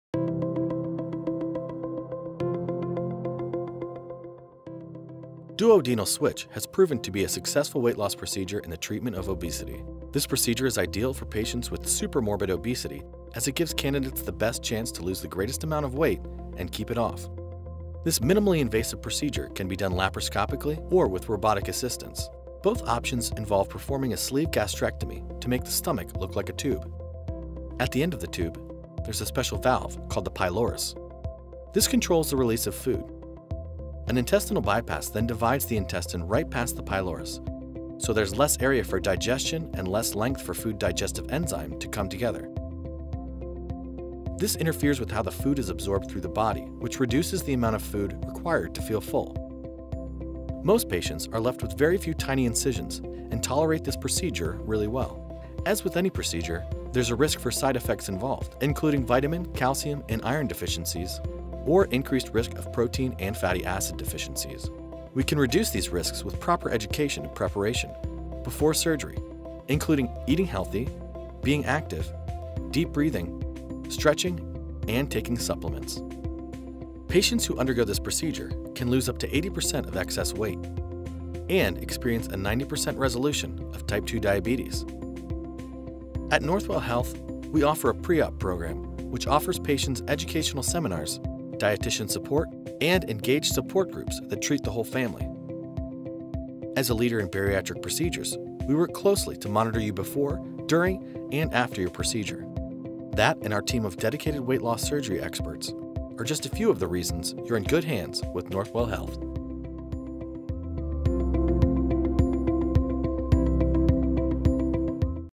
What is a Duodenal Switch?
The duodenal switch is a minimally invasive procedure that combines a sleeve gastrectomy (restriction) with a bypass of the intestines (malabsorption). This procedure is associated with the highest average weight loss, lowest chance of relapsed weight gain, and offers a high resolution of obesity related health problems, like diabetes, hypertension and sleep apnea.